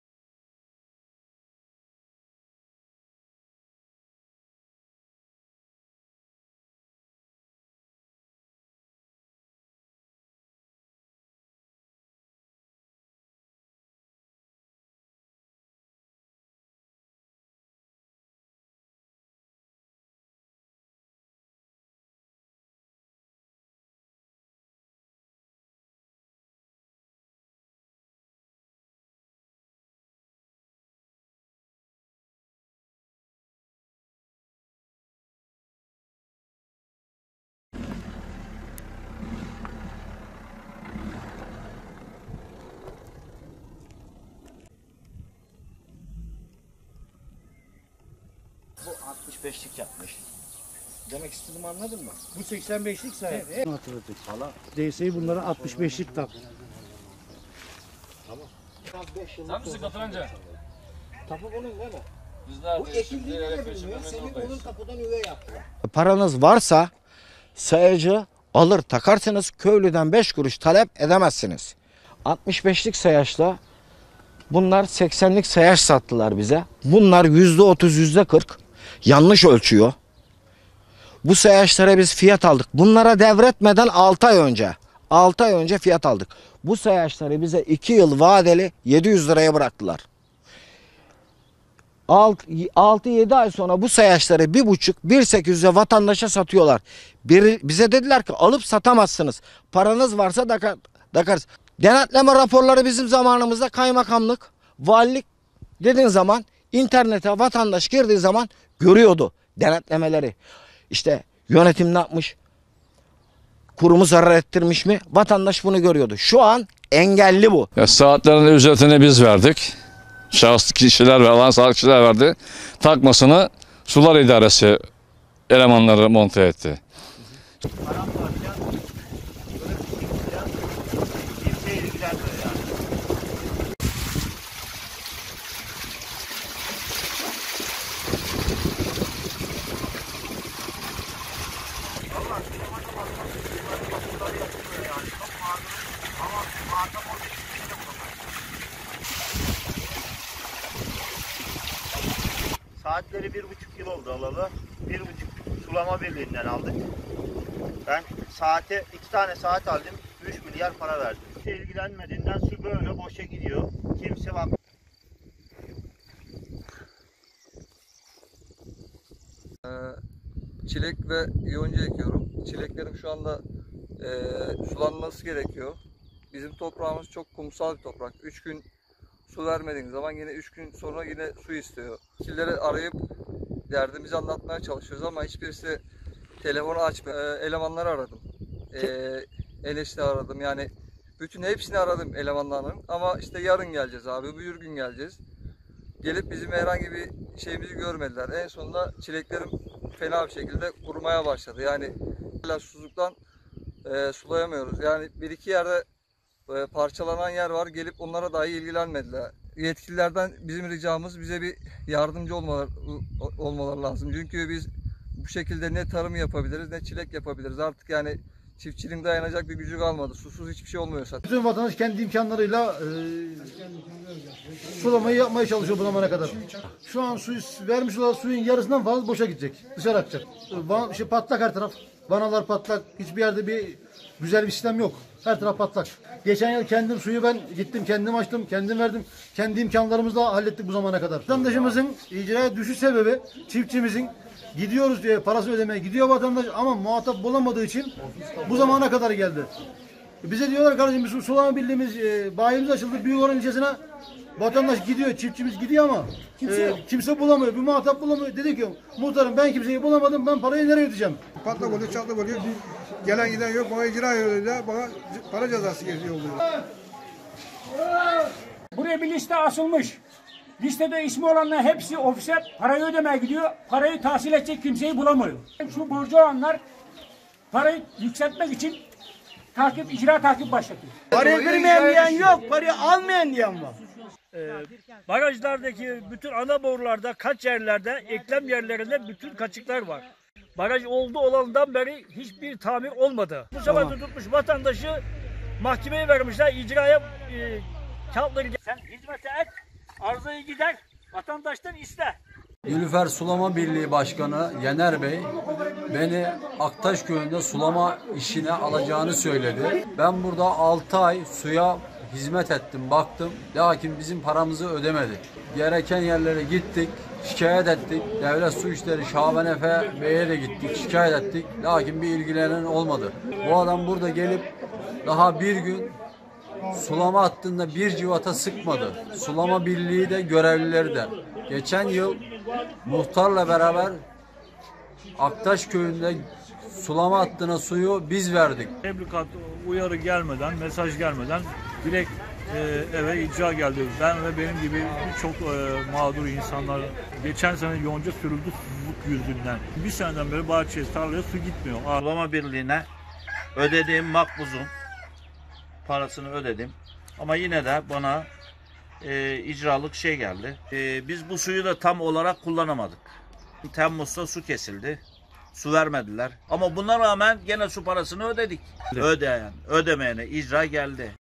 Bursa’da çiftçiler gelen icra ile şaşkına döndü
Bursa’da yüzlerce çiftçi, sulama birliğinden gelen icra ile neye uğradığını şaşırdı.